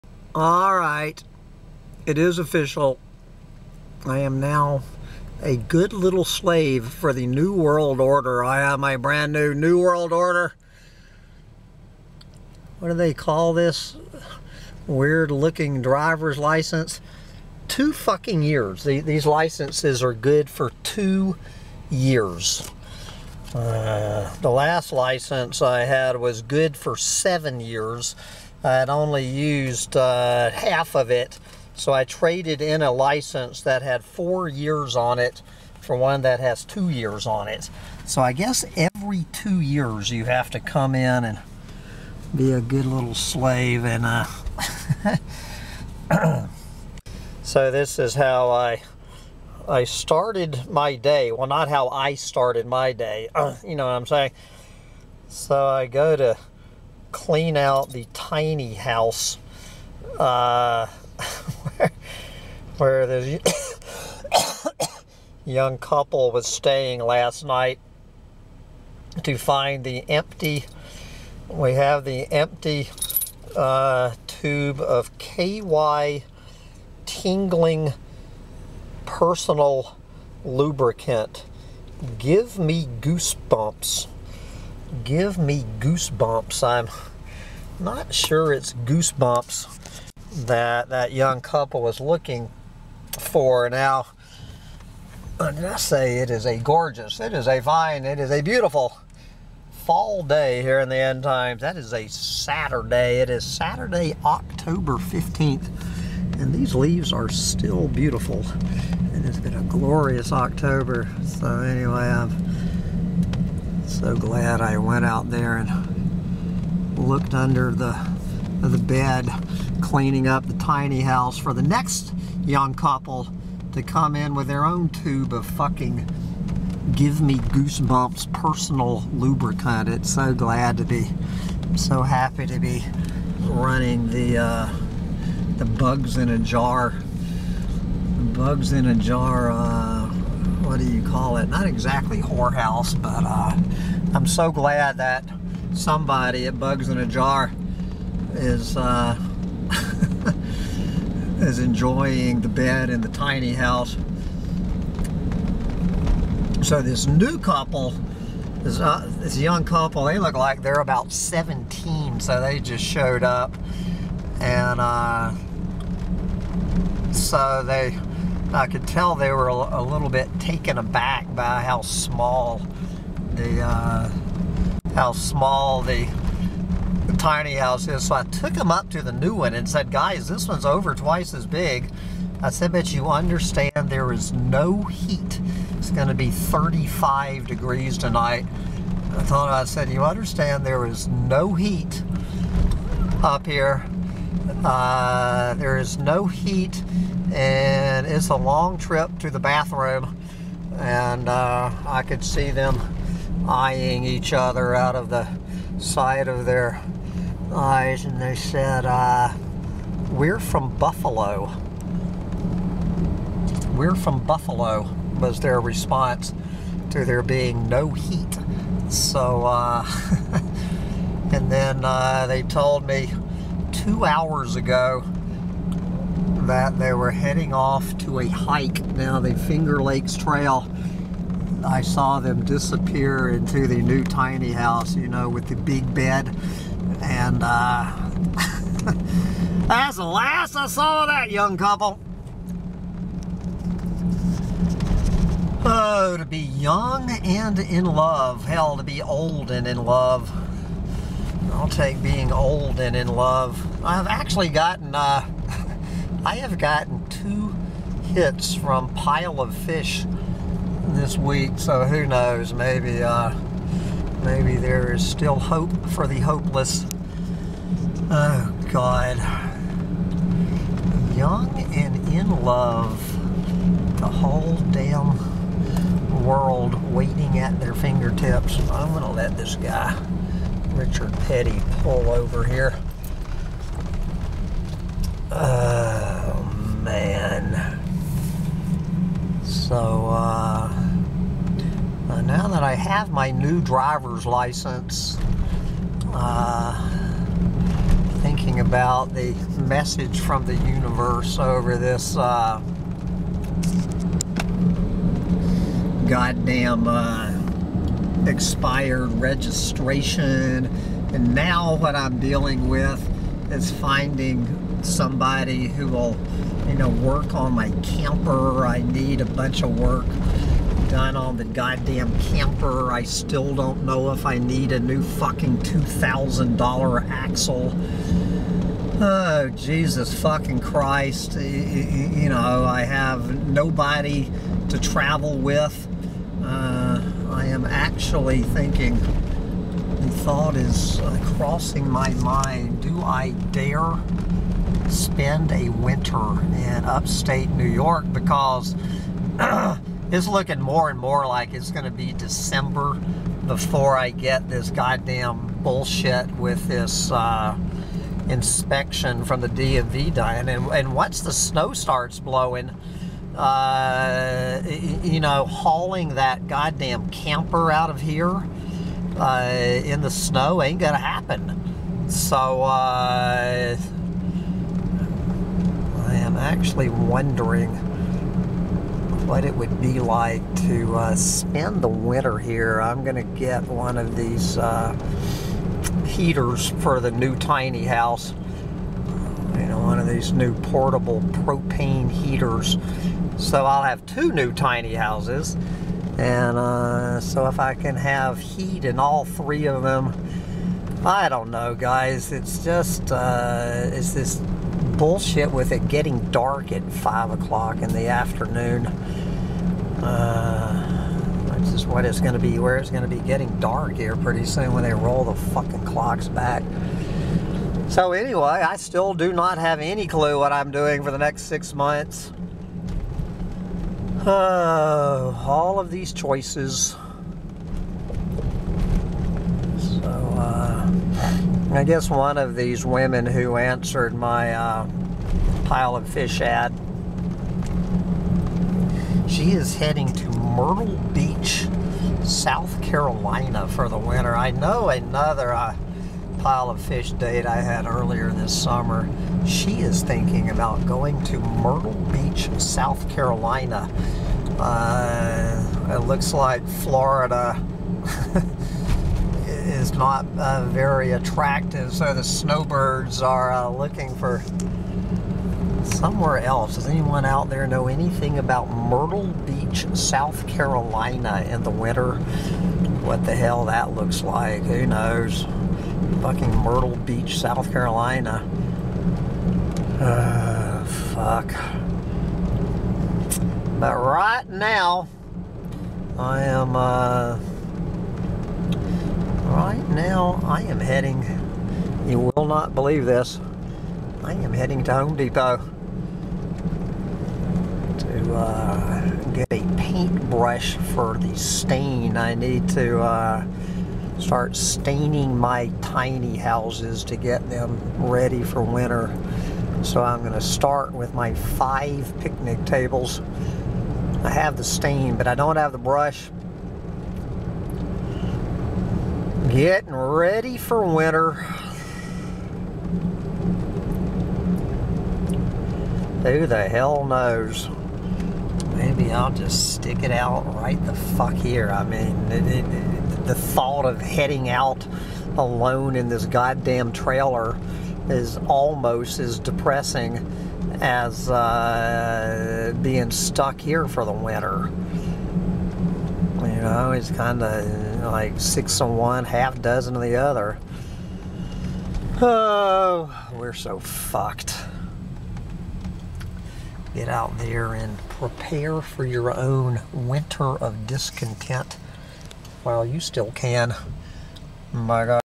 A Depressed Collapsitarian Weighs His Depressing Choices as Old Man Winter Bears Down
I am still trying to figure out the message from the Universe contained in an empty bottle of KY personal lubricant I found in the tiny house this morning, to start out this day.